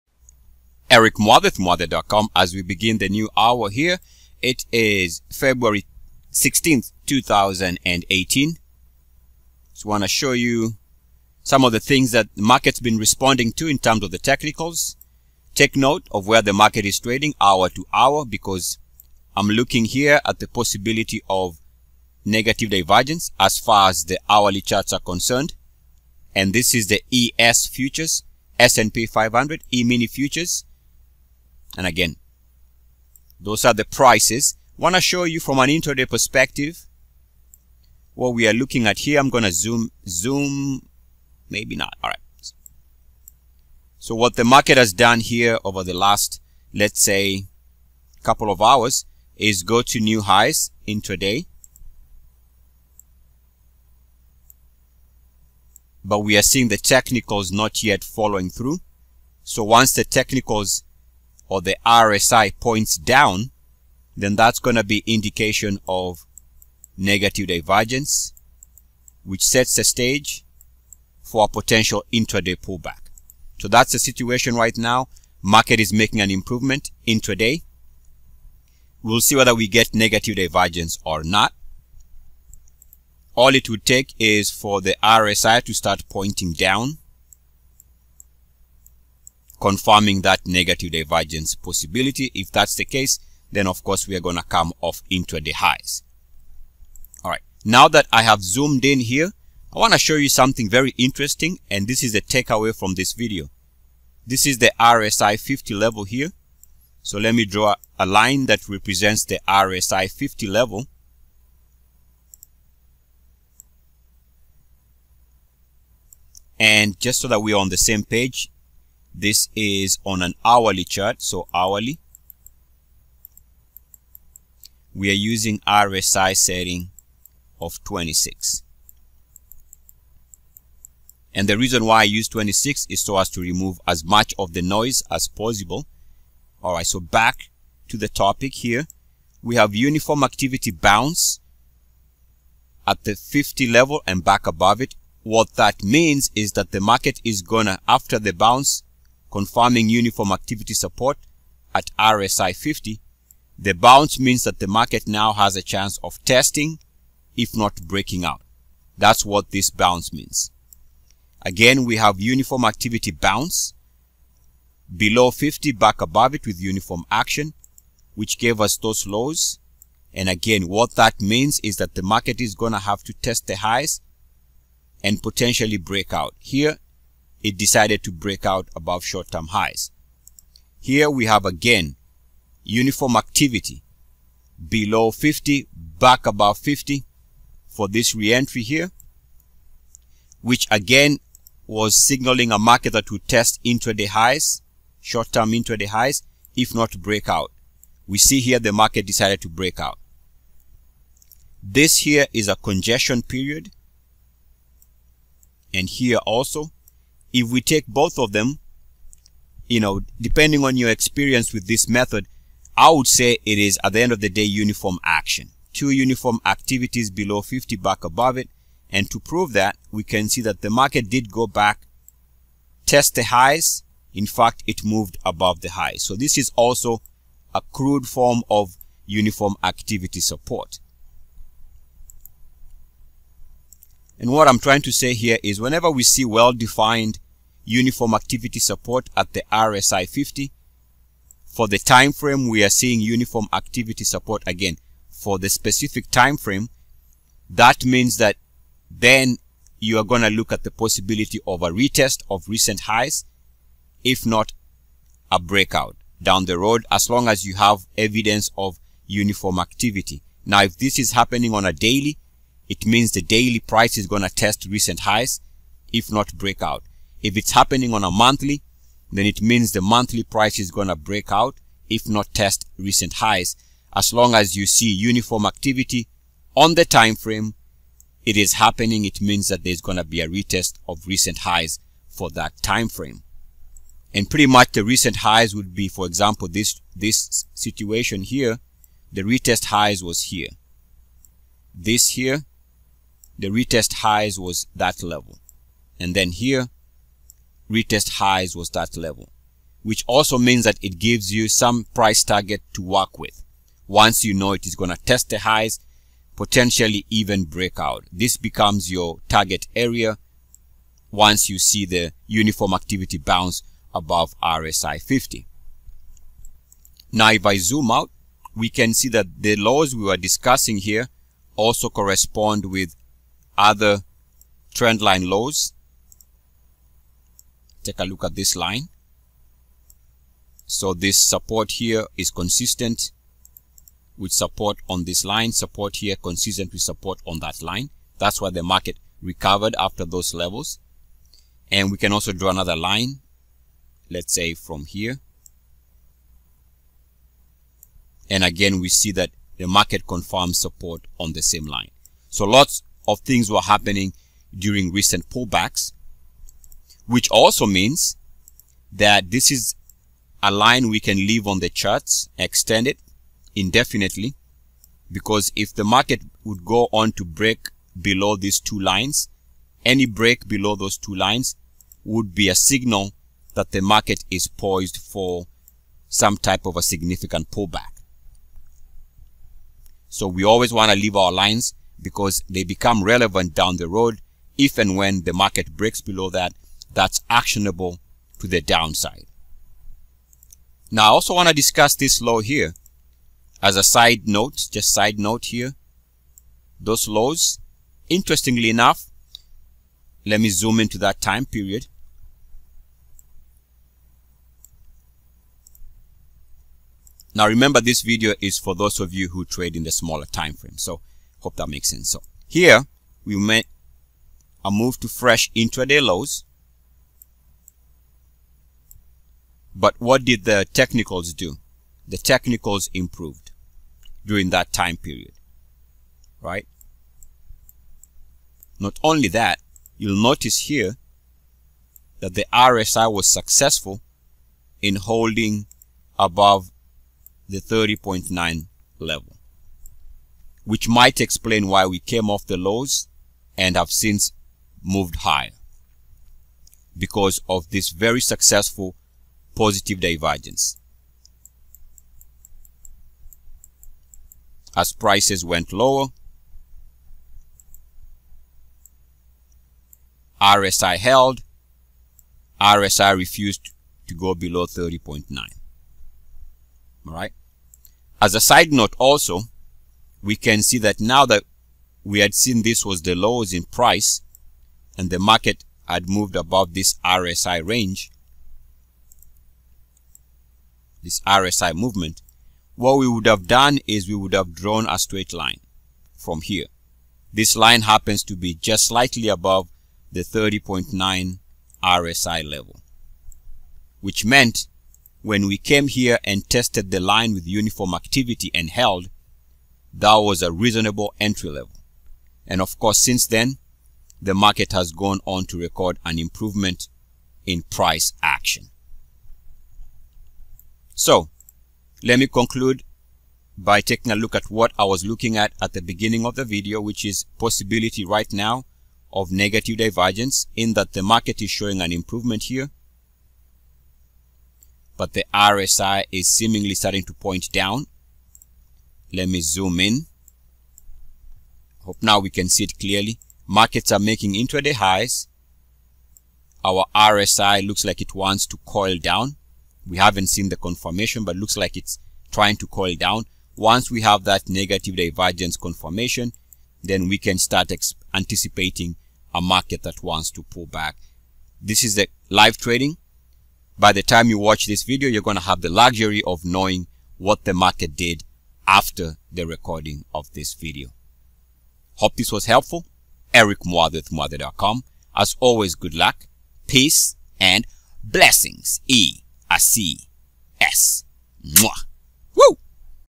Day Trading RSI Uniformity + Divergence - #674
Day trading RSI 50, 30.9 uniformity, negative divergence and more...

Markets are Rigged. Using Charts and Technical Analysis alone is a road to the Poorhouse
The science of technical analysis vs. the art of trading